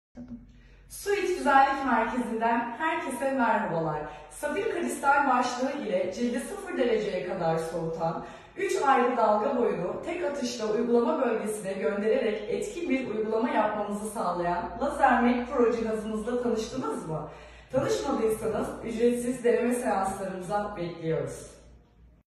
Lasermach Pro Diodelaser hairremoval system
“Say hello to hair-free skin with the Lasermach Pro Diodelaser ”⁣
✅Medical grade hair removal⁣
✅Permanent results⁣
✅Smart diode machines, brand new⁣
✅60% more powerful than the market leader⁣
✅ICE cooling technology for pain-free removal⁣